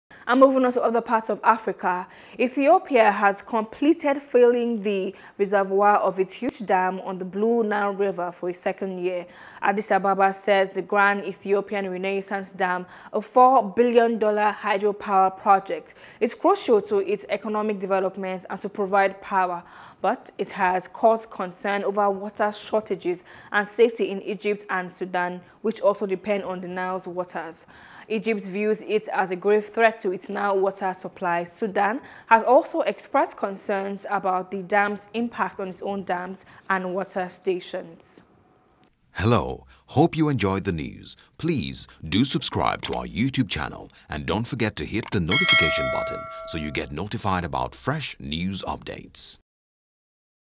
Ethiopia Completes Second Filling Of Grand Renaissance Dam | AFRICAN
Ethiopia has completed filling the reservoir of its huge dam on the Blue Nile river for a second year.